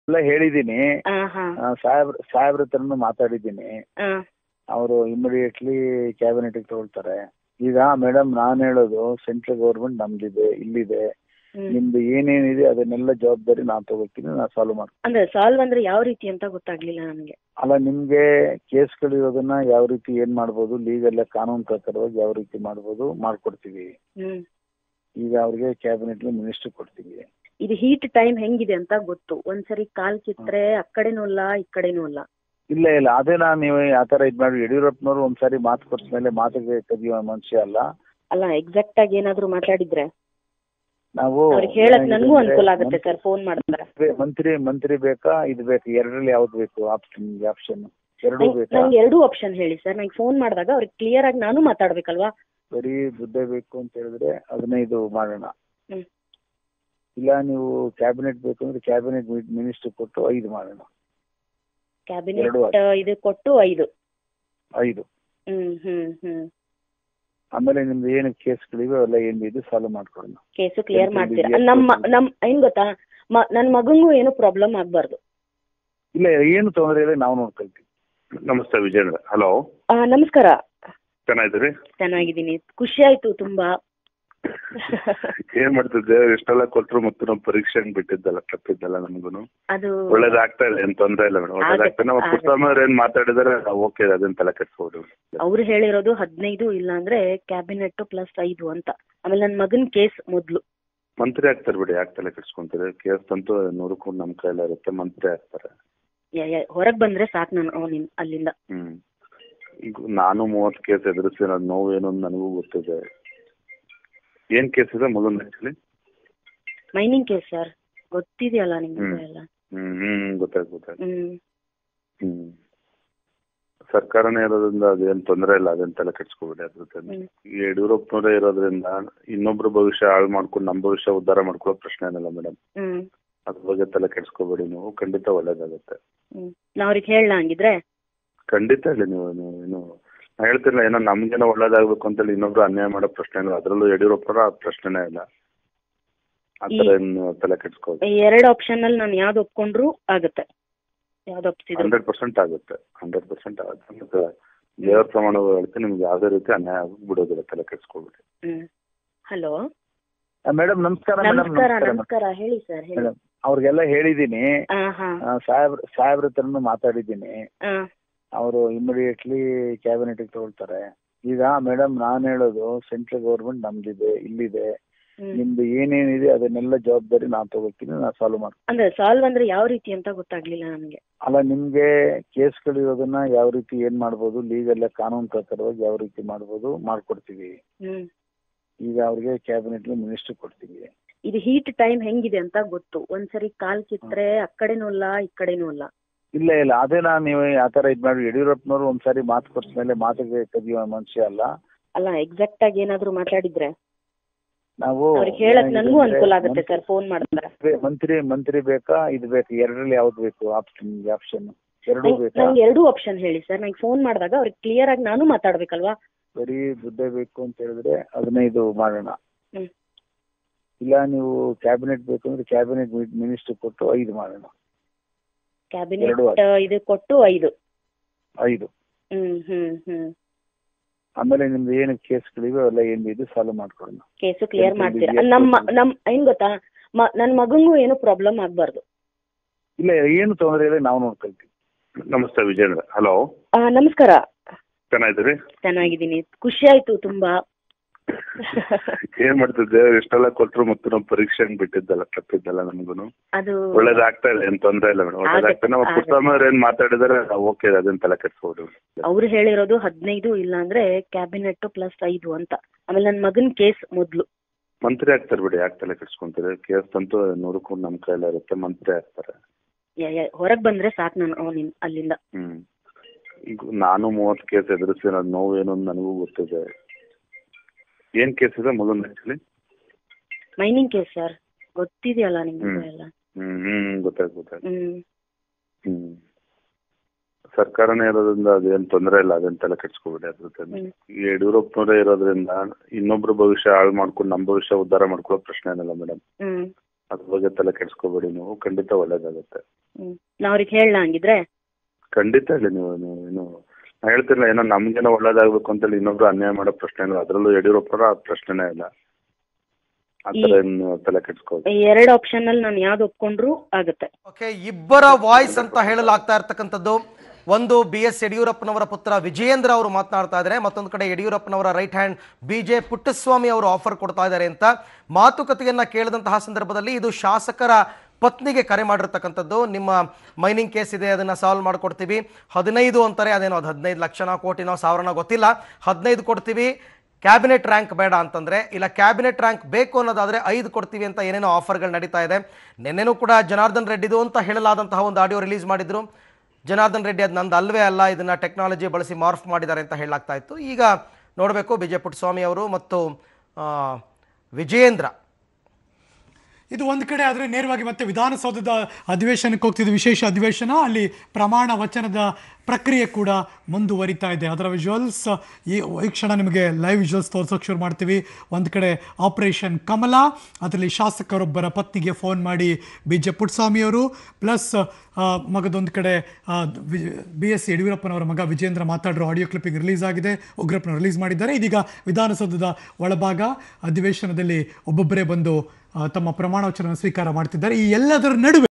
Breaking | BSY's Son & Puttaswamy's Sting Tape To Bribe Cong MLA's Wife Released
CM Yeddyurappa's Son BY Vijayendra  and Puttaswamy have jointly spoken to Congress MLA wife and offered Cabinet post if ready to join BJP.